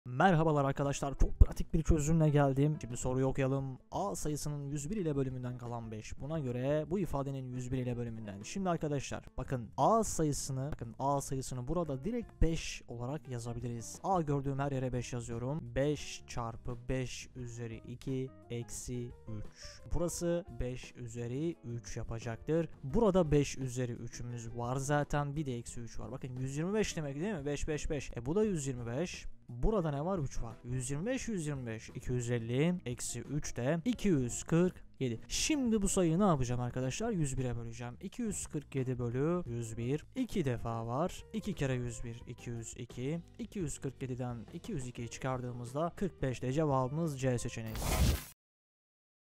bölme bölünebilme pratik çözüm
Arkadaşlar Merhaba tugay hoca ben. Bugün bölünebilme çözdük. Günlük taze matematik soruları için takipte kalmaya devam edelim. Soru ve not paylaşımları için instagram adresim